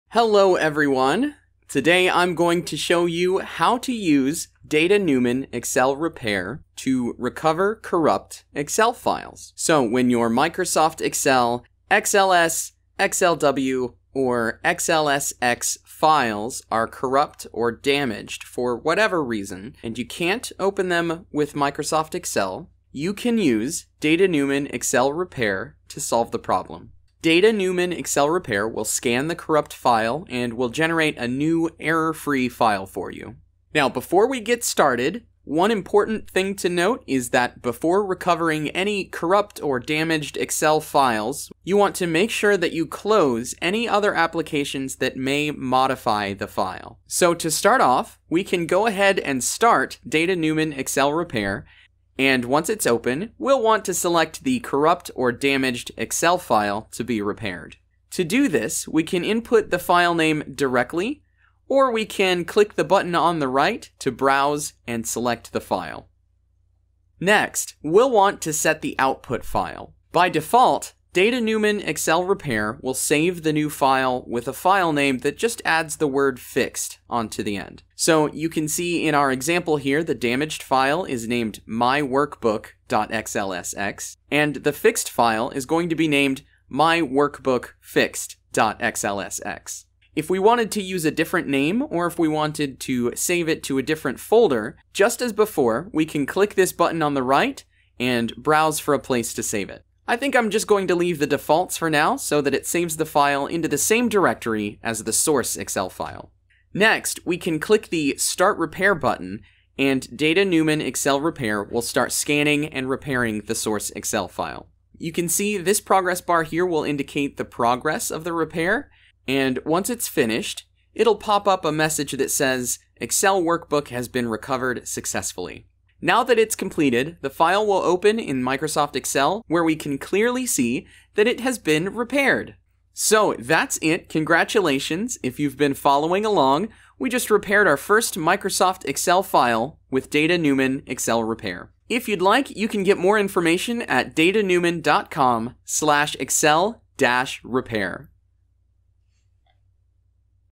How to Use DataNumen Excel Repair to Recover Corrupt Excel Files
In this tutorial, we will provide a step by step guide on how to use DataNumen Excel Repair to recover corrupt Excel .xls, .xlw and .xlsx files. for details.